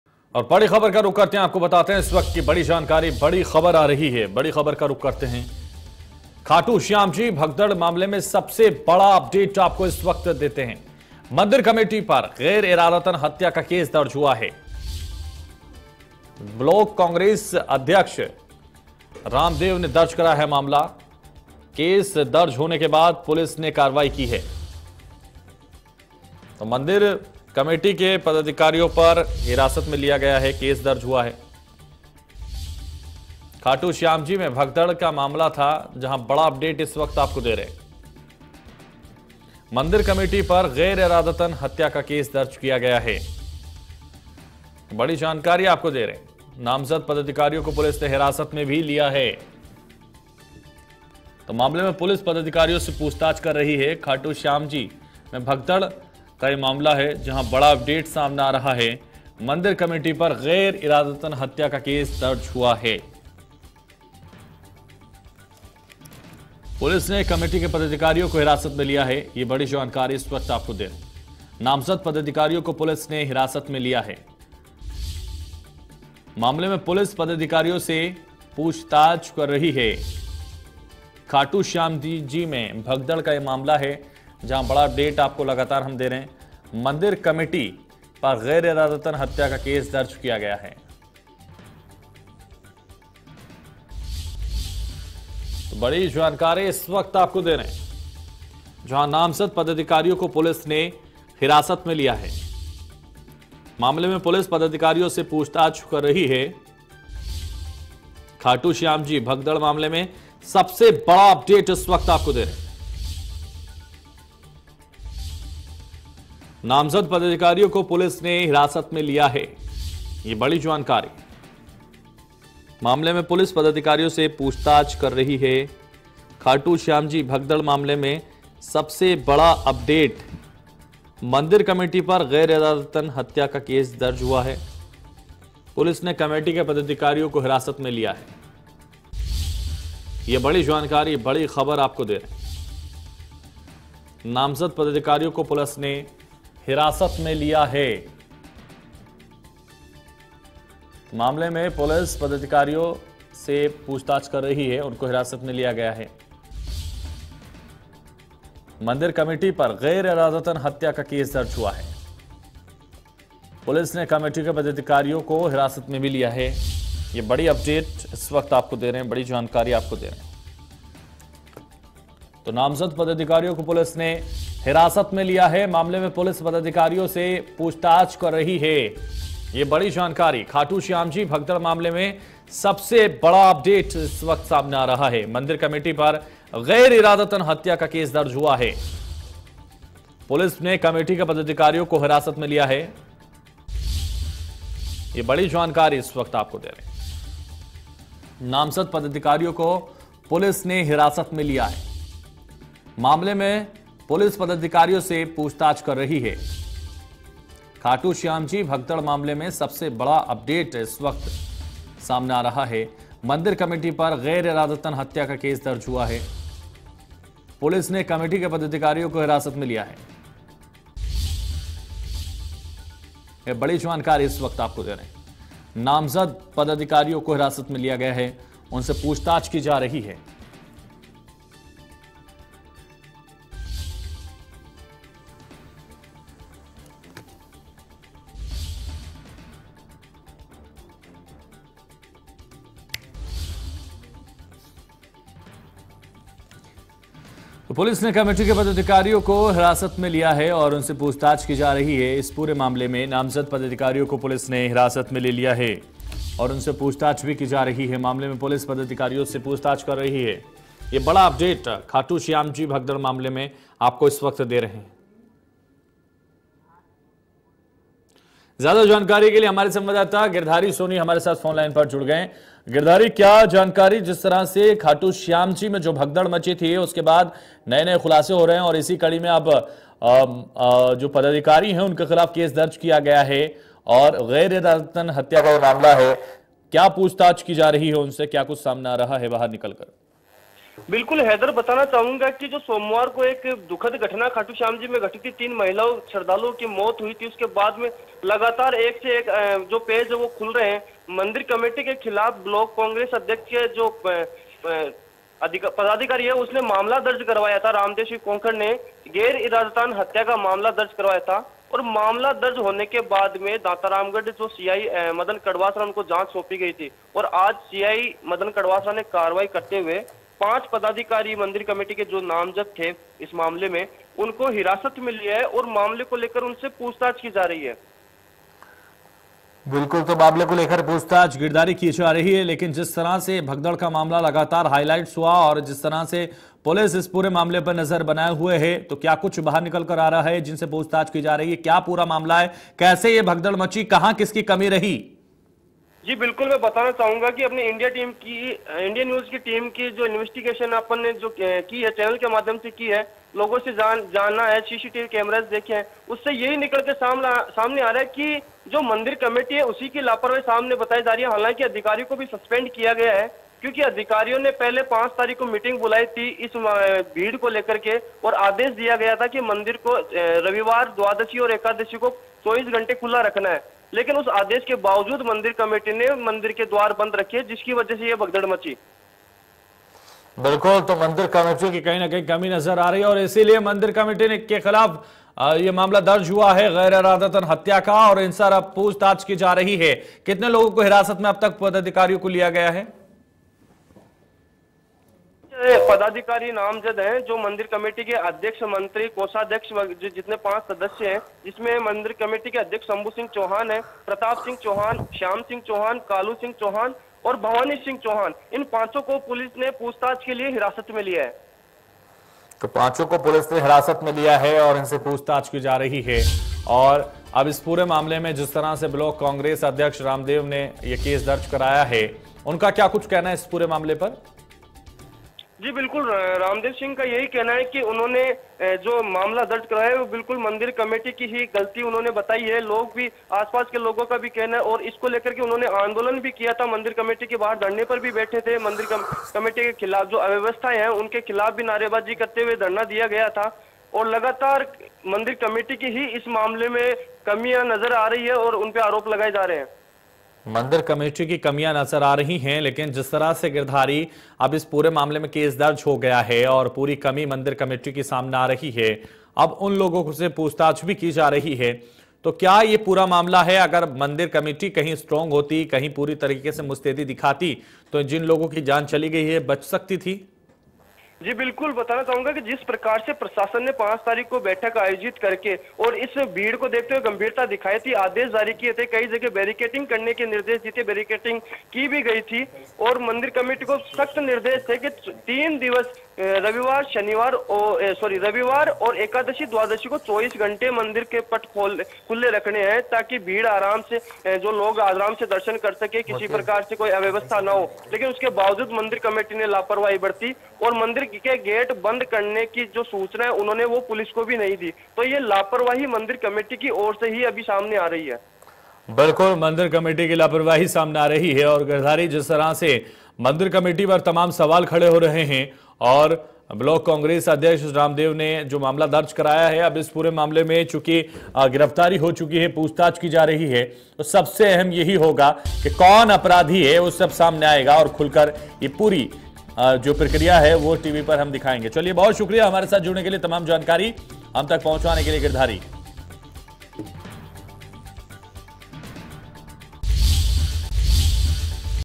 Khatu Shyam Temple : खाटूश्यामजी भगदड़ मामले में सबसे बड़ा अपडेट | India News Rajasthan
Khatu Shyam Temple : खाटूश्यामजी भगदड़ मामले में सबसे बड़ा अपडेट, मंदिर कमेटी के पदाधिकारियों को हिरासत में  लिया | Hindi News